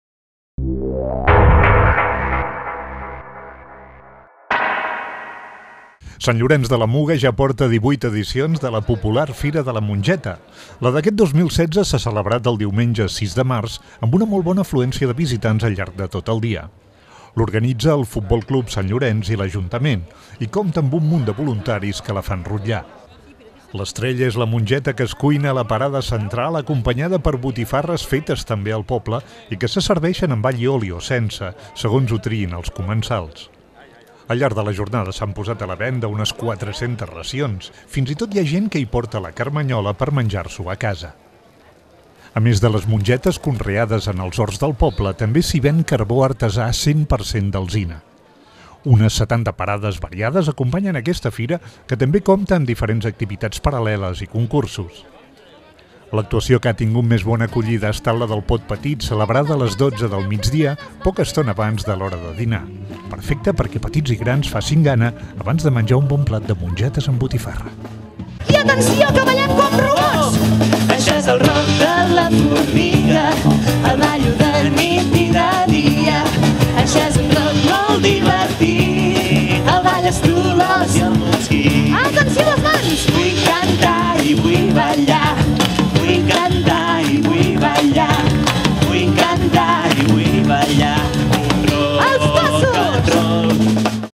Fira de la Mongeta 2016 a Sant Llorenç de la Muga
La divuitena edició de la Fira de la Mongeta de Sant Llorenç de la Muga ha comptat amb una molt bona afluència de visitants. S'hi han servit mongetes amb botifarres i hi ha hagut moltes activitats paral·leles com l'actuació del grup El Pot Petit.